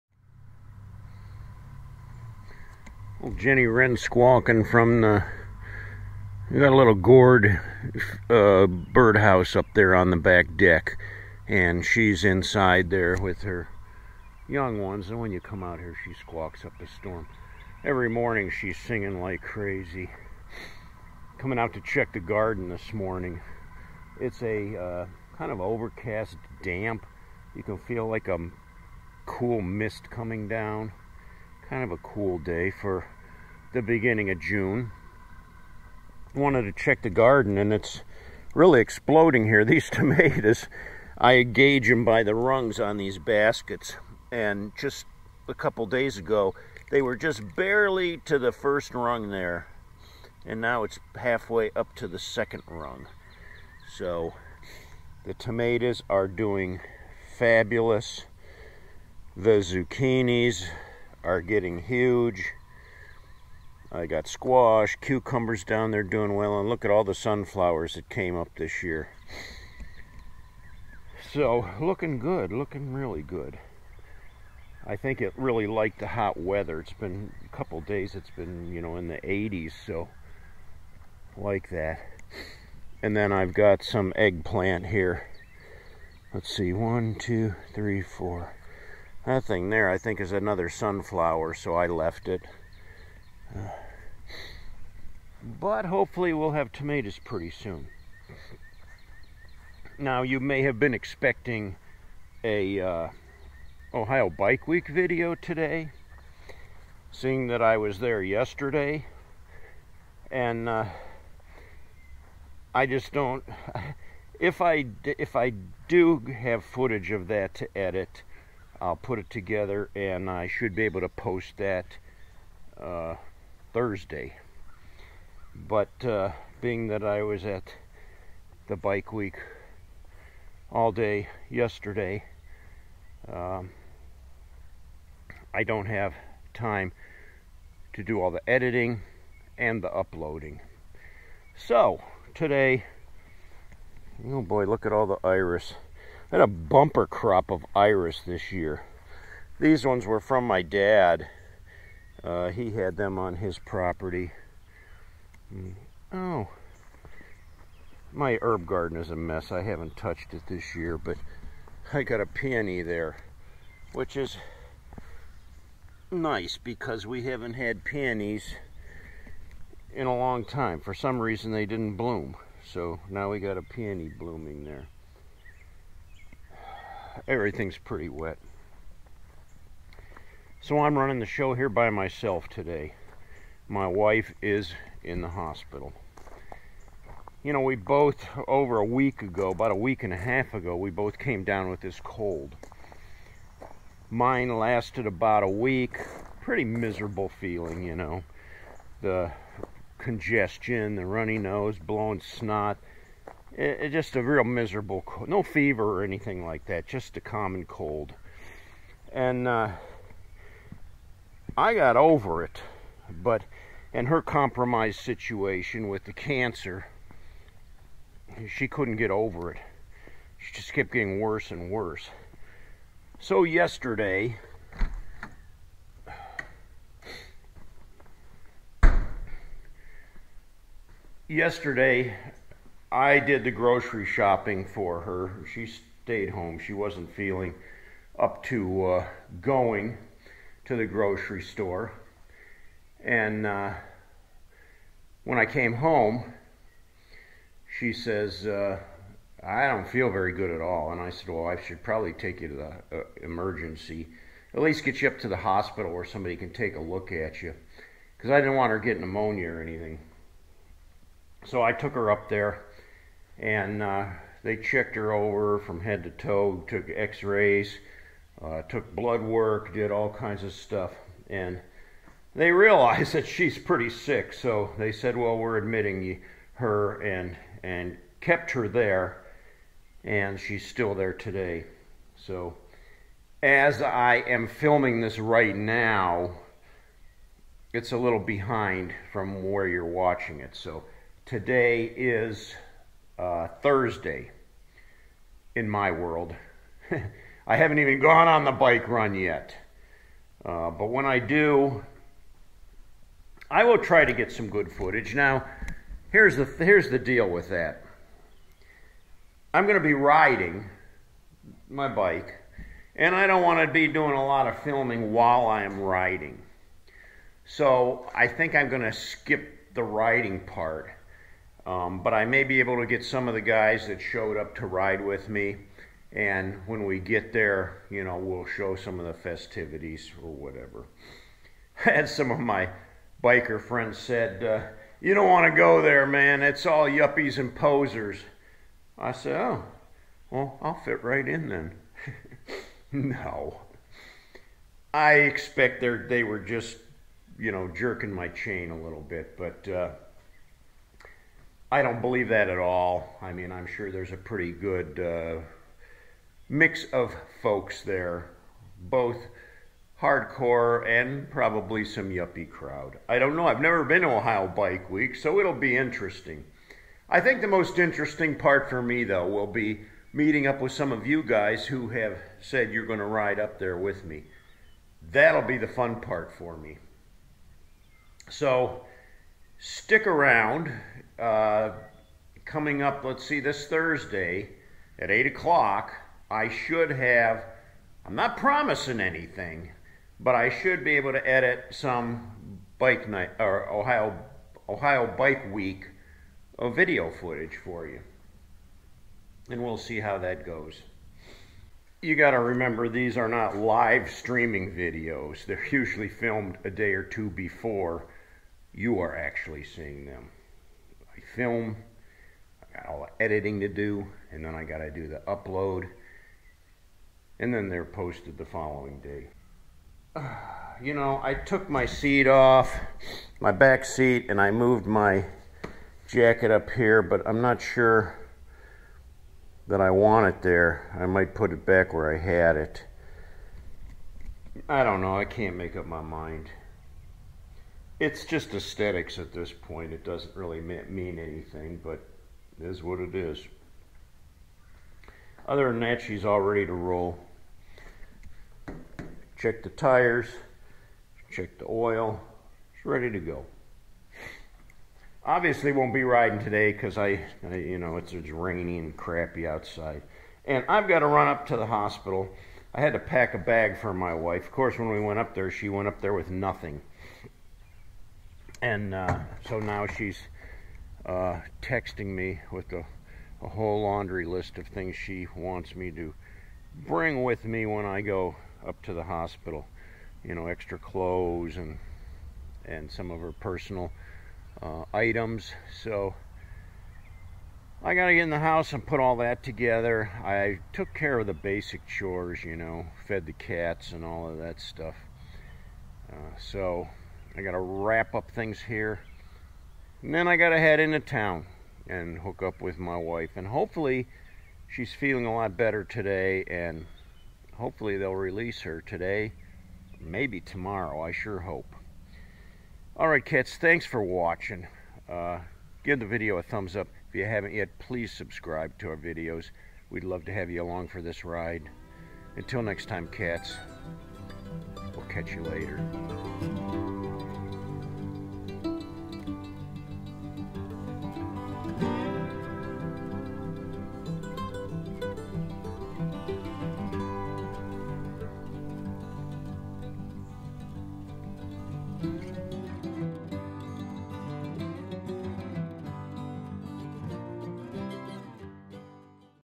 Running Solo
Itchy shows off the gardens, talks about his upcoming trip to Ohio Bike Week, and then must care for Vee as she's quite sick and in the hospital.